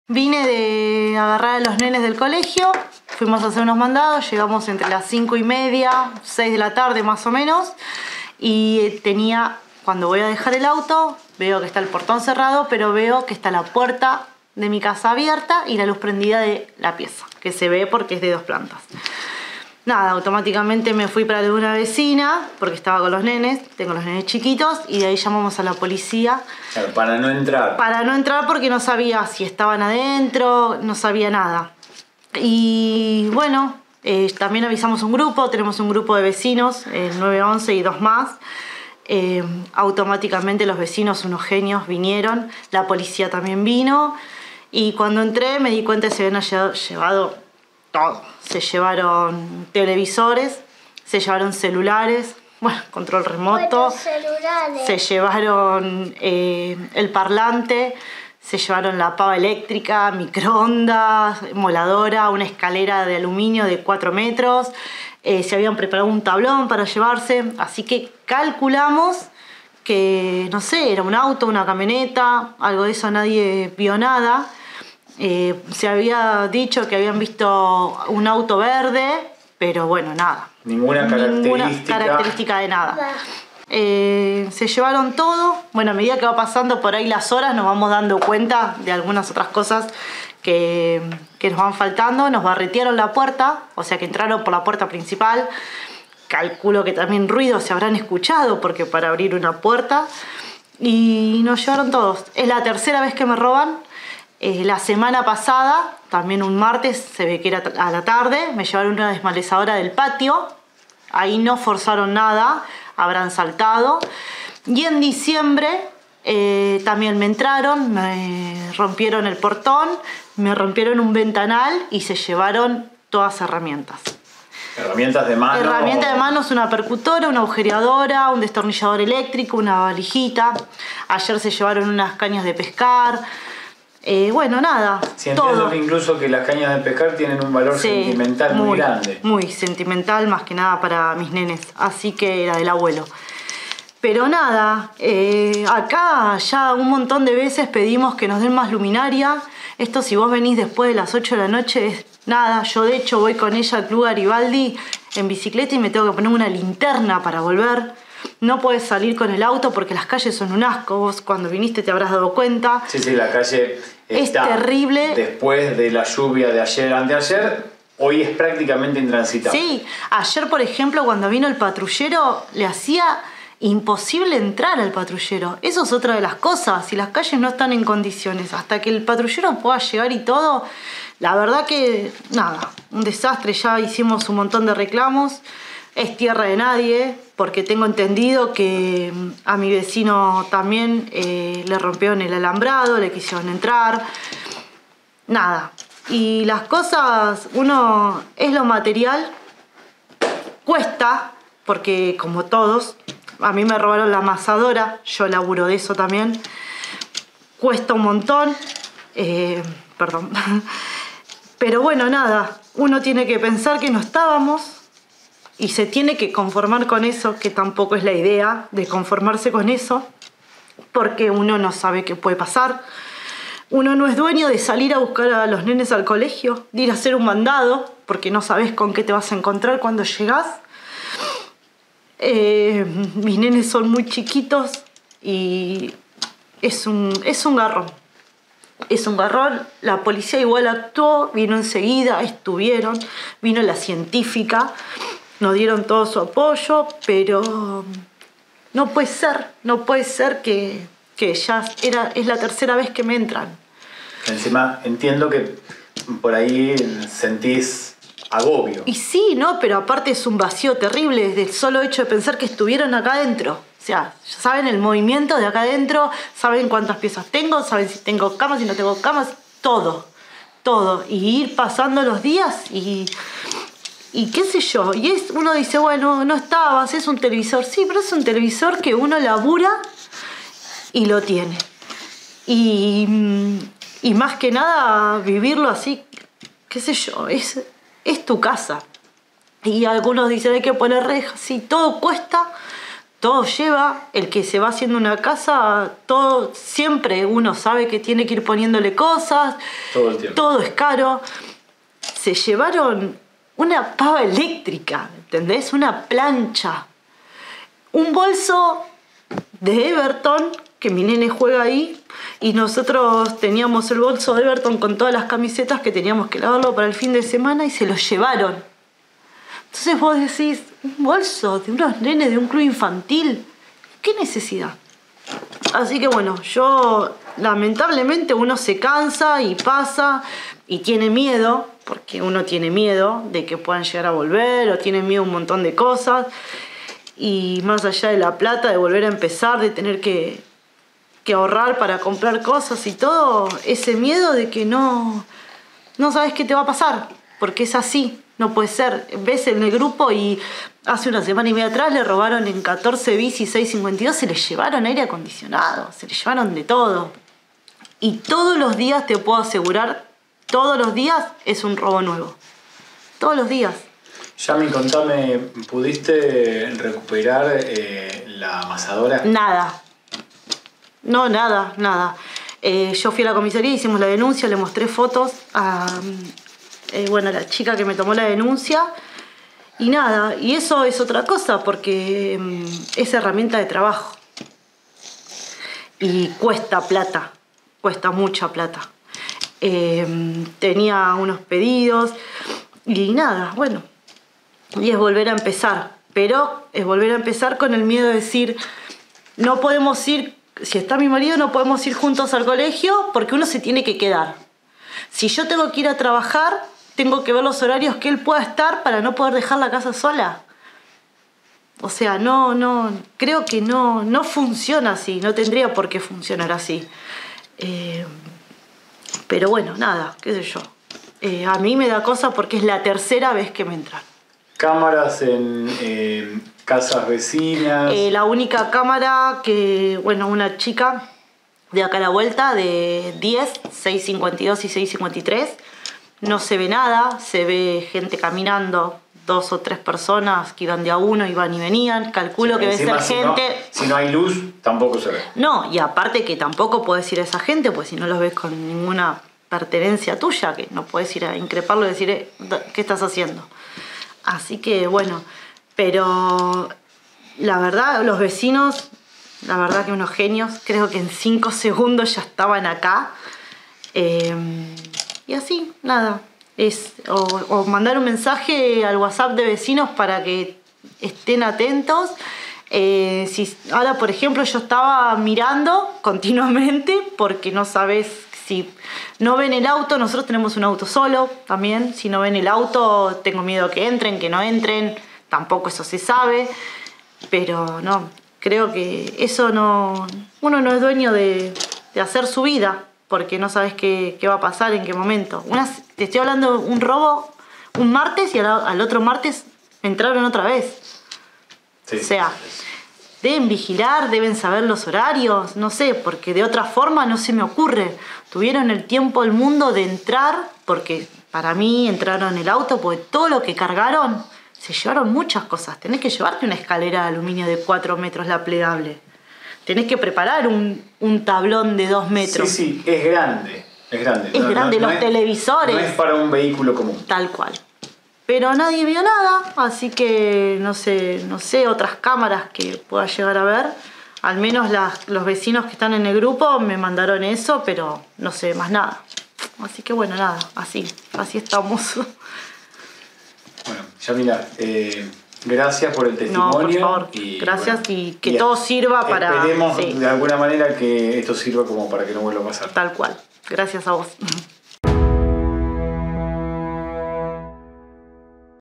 Sufrió tres robos en los últimos cuatro meses
Este martes, Yamila volvió a su casa y descubrió habían forzado su puerta de entrada y se llevaron varios elementos, desde televisores hasta las cañas de pescar de los niños que era regalo de su abuelo. La semana pasada había sido víctima de otro hecho.